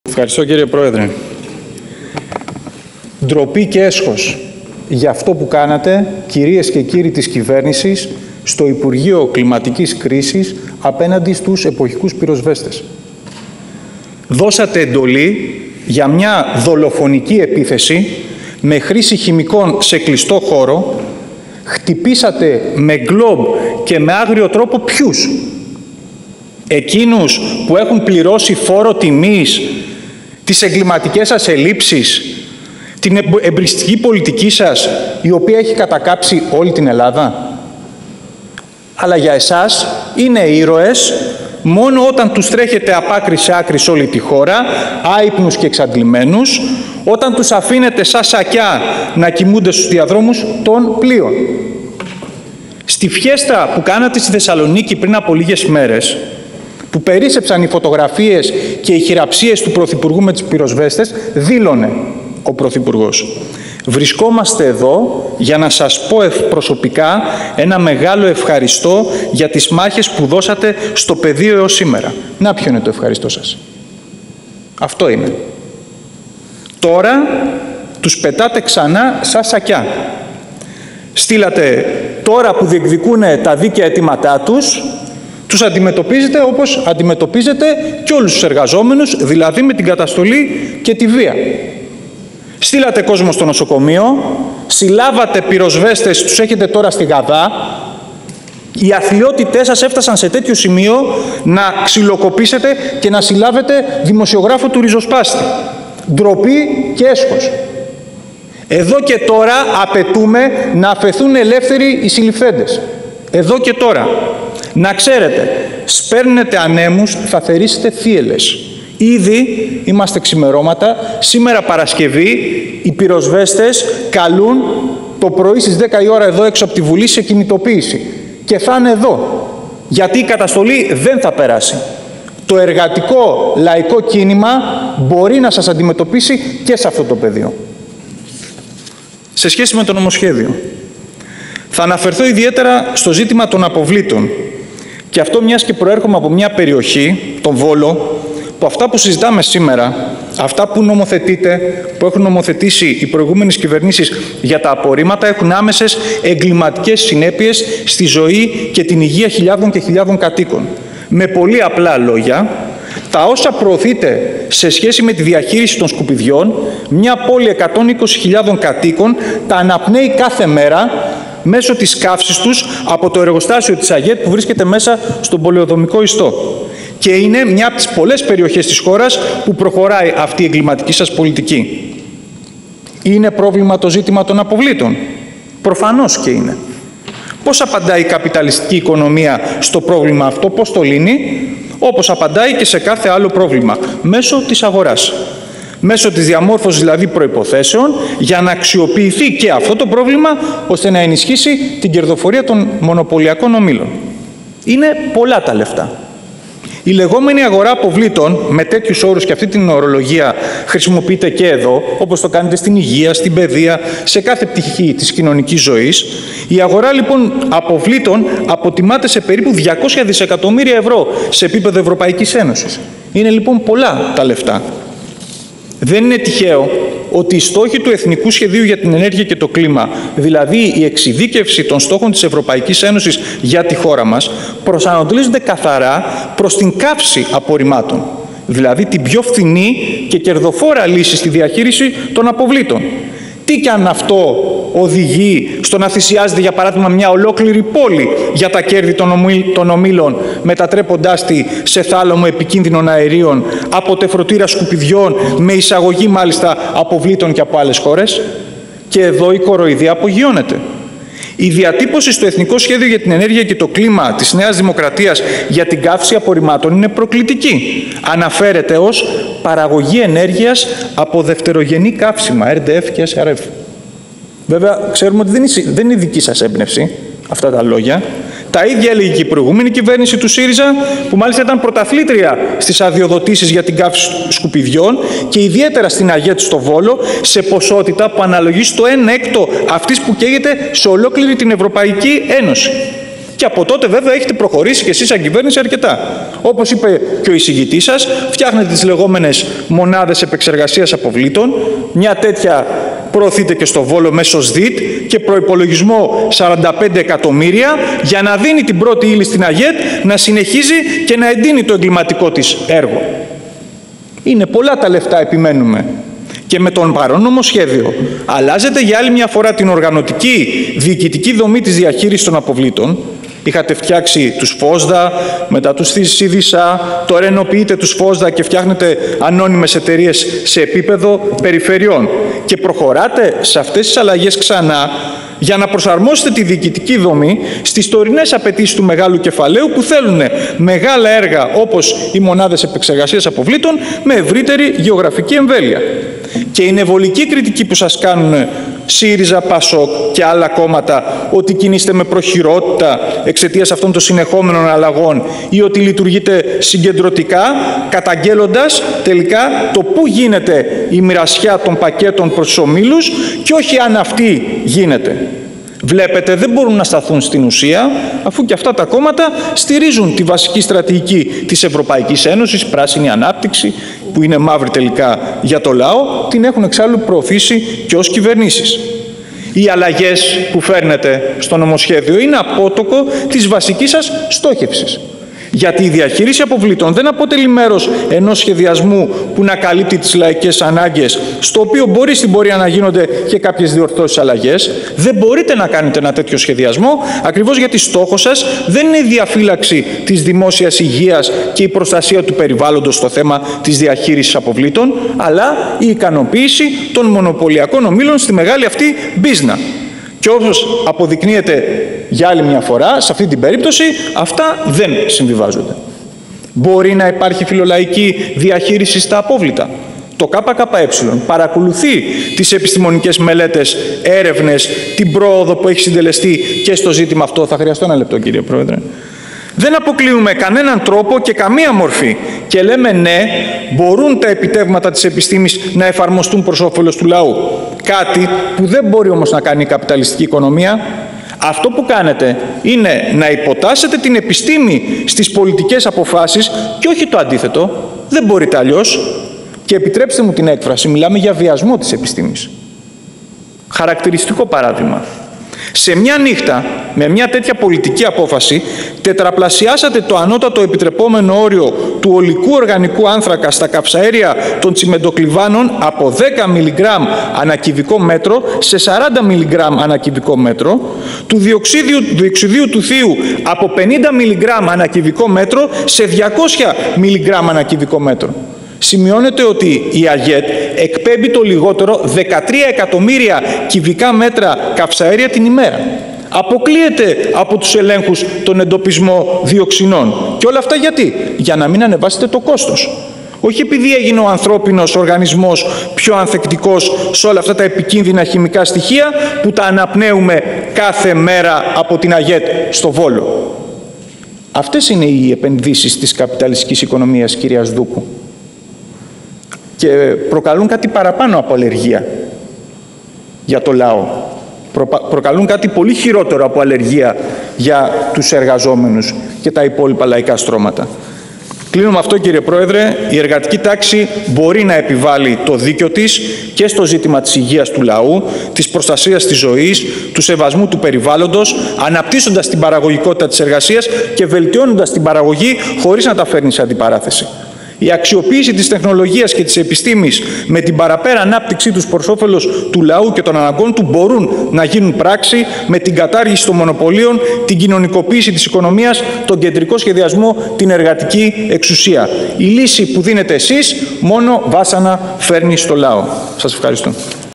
Β. Μεταξάς: Στόχος σας η ικανοποίηση μονοπωλίων από τη διαχείριση αποβλήτων
Στις εγκληματικές συνέπειες όσων νομοθετούνται για τη διαχείριση των αποβλήτων στη ζωή και στην υγεία του λαού και κυρίως της Θεσσαλίας, στάθηκε ο *Βασίλης Μεταξάς,* βουλευτής του ΚΚΕ, μιλώντας στην Ολομέλεια της Βουλής, στο πλαίσιο της χθεσινής συζήτησης για το νομοσχέδιο του υπουργείου Περιβάλλοντος και Ανάπτυξης.

Παρουσίασε το χαρακτηριστικό παράδειγμα της εγκληματικής πολιτικής που βιώνουν οι κάτοικοι του Βόλου εξαιτίας της καύσης σκουπιδιών στο εργοστάσιο της ΑΓΕΤ. Επισήμανε, ακόμα, ότι επειδή «παίζονται» πολλά λεφτά στην αγορά αποβλήτων, η οποία αποτιμάται στα 200 δισ. ευρώ σε επίπεδο Ευρωπαϊκής Ένωσης, οι στόχοι του Εθνικού Σχεδίου για την Ενέργεια και το Κλίμα (ΕΣΕΚ), δηλαδή η εξειδίκευση των σχετικών στόχων της ΕΕ για την Ελλάδα, προσανατολίζονται στην καύση απορριμμάτων και ταυτόχρονα στην πιο φθηνή και κερδοφόρα διαχείριση αποβλήτων. Αναφερόμενος στην προκλητική αναφορά της κυβέρνησης της ΝΔ στο ΕΣΕΚ περί «παραγωγής Ενέργειας από δευτερογενή καύσιμα», υπενθύμισε ότι τα ίδια έλεγε και η κυβέρνηση ΣΥΡΙΖΑ.

Ανέδειξε, επίσης, το γεγονός ότι η κυβέρνηση προωθεί στον Βόλο μέσω ΣΔΙΤ μονάδες επεξεργασίας αποβλήτων, μία τέτοια προωθείται και στον Βόλο μέσω ΣΔΙΤ «για να δίνει την πρώτη ύλη στην ΑΓΕΤ να συνεχίζει και να εντείνει το εγκληματικό της έργο».

Χαρακτήρισε «βολική» την κριτική των άλλων κομμάτων περί προχειρότητας εξαιτίας συνεχόμενων αλλαγών, ακριβώς γιατί στηρίζουν την ευρωενωσιακή πράσινη ανάπτυξη και την έχουν προωθήσει και ως κυβερνήσεις. «Οι αλλαγές που φέρνετε είναι απότοκο της βασικής σας στόχευσης», εξήγησε ο Β. Μεταξάς και πρόσθεσε: «Στόχος σας δεν είναι η διαφύλαξη της δημόσιας υγείας και η προστασία του περιβάλλοντος στο θέμα της διαχείρισης αποβλήτων, αλλά η ικανοποίηση των μονοπωλίων στη μεγάλη αυτή μπίζνα».

Ο Β. Μεταξάς, μιλώντας για τη φιλολαϊκή διαχείριση των αποβλήτων, υπογράμμισε ότι το ΚΚΕ λέει «ναι» στην εφαρμογή των επιστημονικών επιτευγμάτων προς όφελος του λαού. Ανέδειξε τη δυνατότητα οι εργαζόμενοι να επιβάλλουν το δίκιο τους και στο ζήτημα της υγείας, της προστασίας της ζωής του λαού και του σεβασμού του περιβάλλοντος και συμπλήρωσε: «Η αξιοποίηση της τεχνολογίας και της επιστήμης με την παραπέρα ανάπτυξή τους προς όφελος του λαού και των αναγκών του μπορούν να γίνουν πράξη με την κατάργηση των μονοπωλίων, την κοινωνικοποίηση της οικονομίας, τον κεντρικό σχεδιασμό, την εργατική εξουσία».

Τέλος, να σημειωθεί ότι ο βουλευτής του ΚΚΕ στην αρχή της ομιλίας του κατήγγειλε τη δολοφονική επίθεση που δέχθηκαν οι εποχικοί πυροσβέστες στο υπουργείο Πολιτικής Προστασίας και Κλιματικής Κρίσης με γκλοπ και χρήση χημικών σε κλειστό χώρο, όπως και τις συλλήψεις πυροσβεστών και του δημοσιογράφου του «Ριζοσπάστη», απαιτώντας να αφεθούν ελεύθεροι.